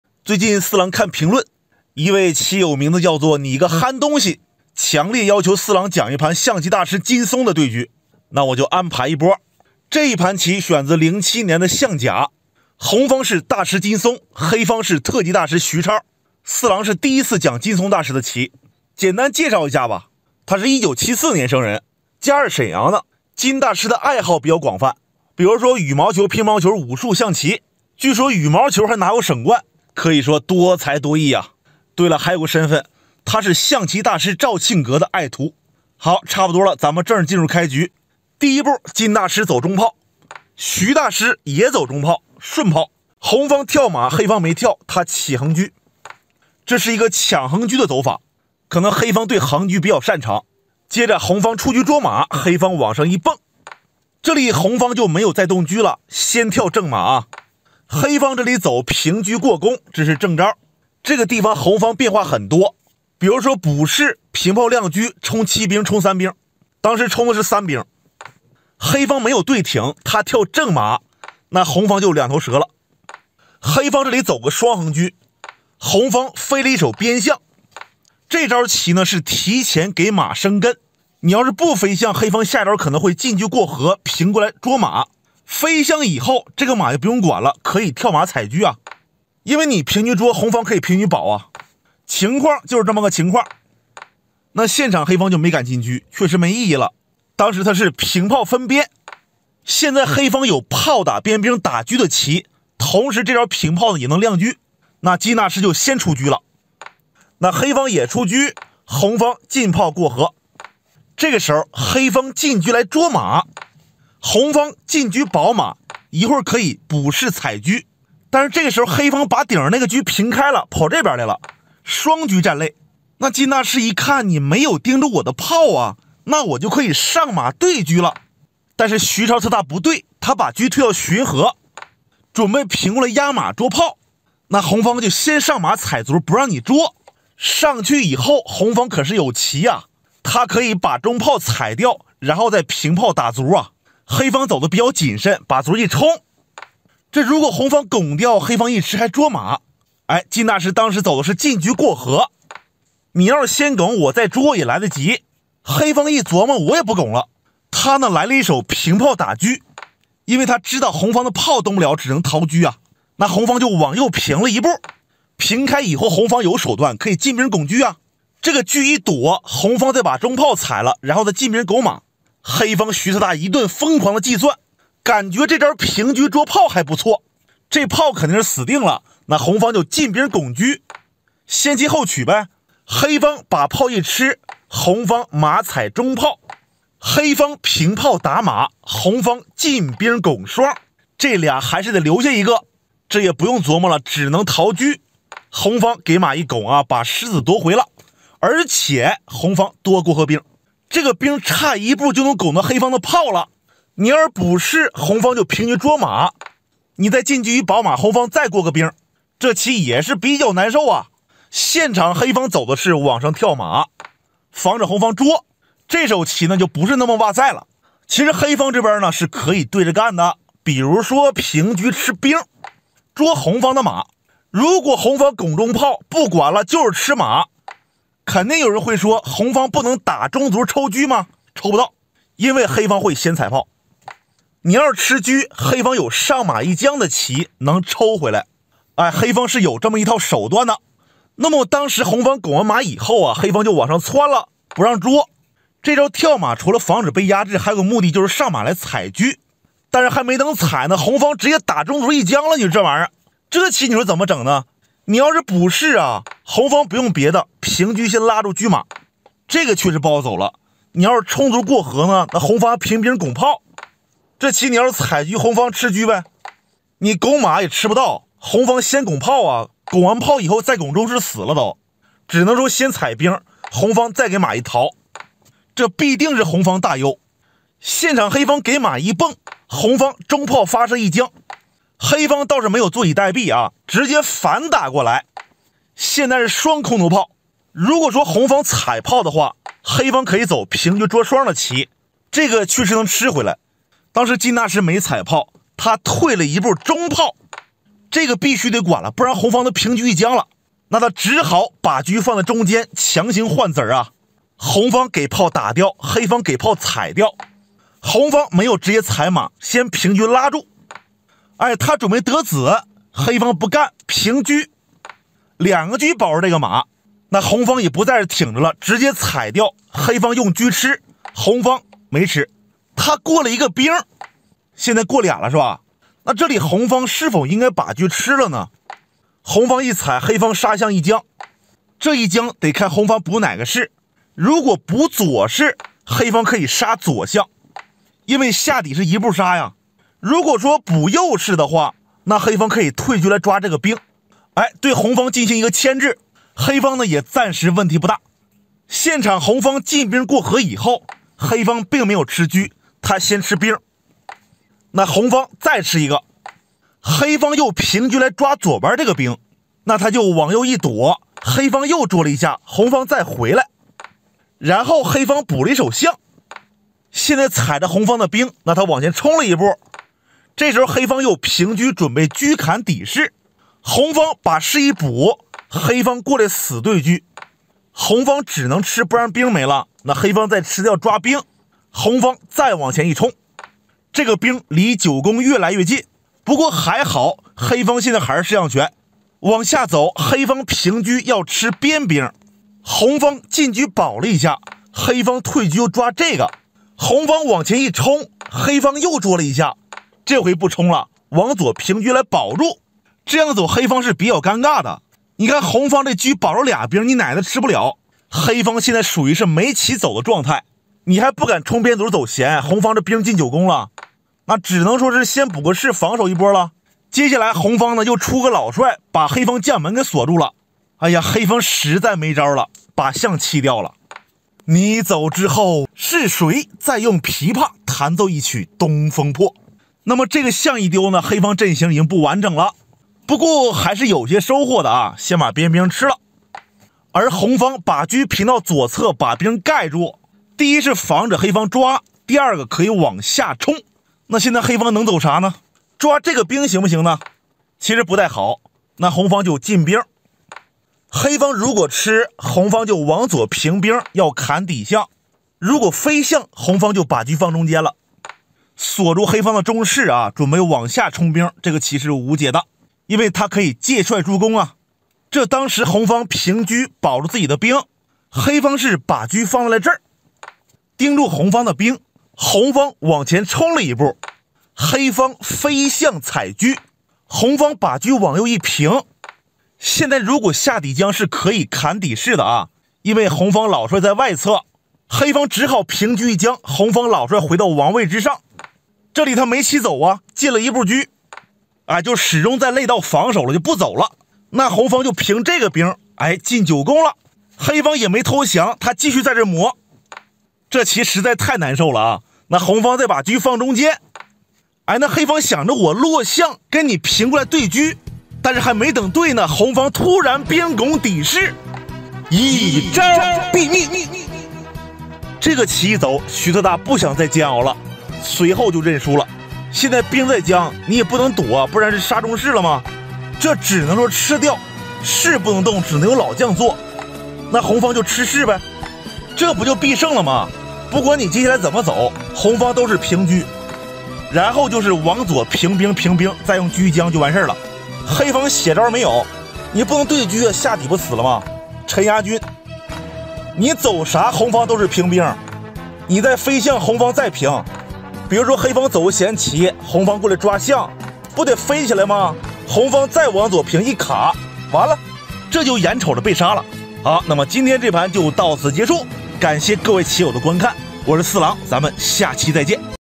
金松vs徐超，象棋大师会武术，特级大师也挡不住【四郎讲棋】
象棋 象棋对战 金松 徐超 请注意，本视频完全符合YouTube社区准则，没有任何违规行为。